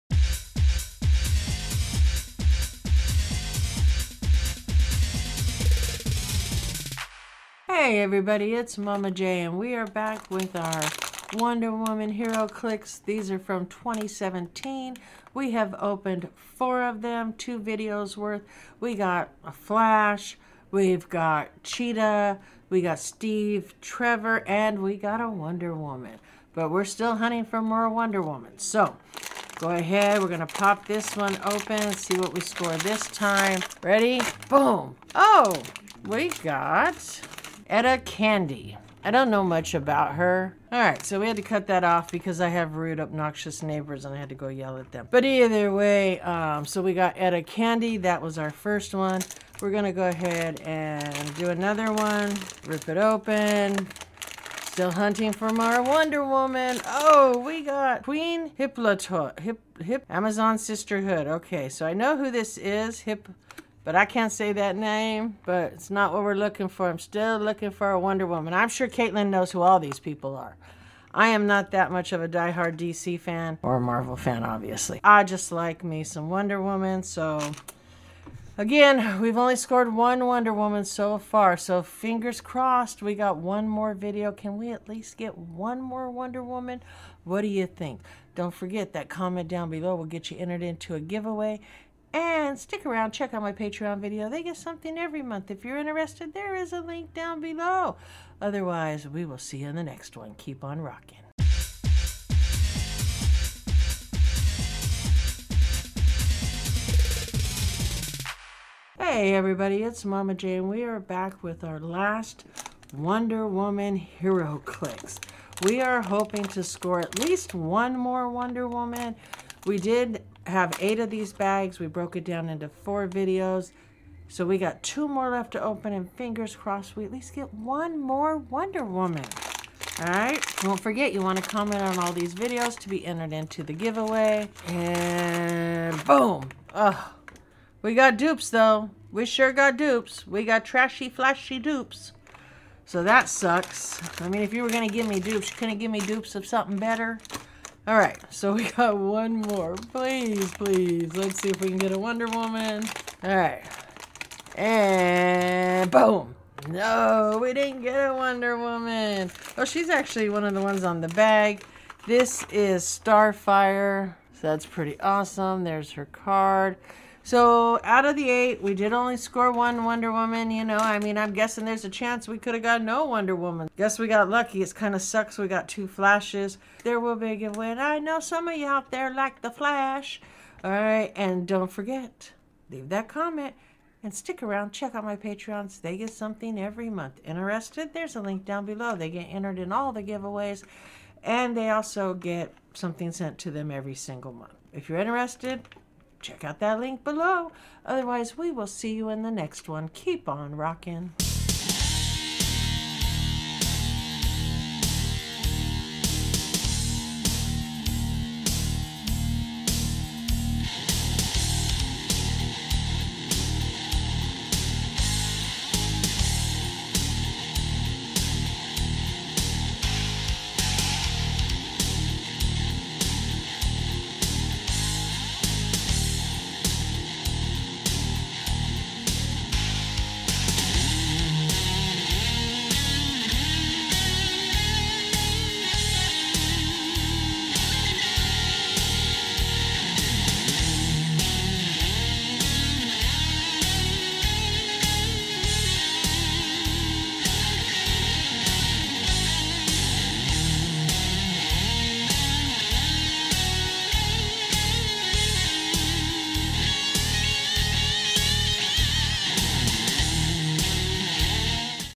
WizKids DC HeroClix: Wonder Woman RIP open 2 bags hunting for WONDER WOMAN part 3
Wonder Woman comes to the HeroClix tabletop along with her trusted allies and troublesome enemies! Deflect bullets, extract truth, and KO your enemies with the DC Comics HeroClix

IG CHANNELS
@MAMA_J_1970 basic IG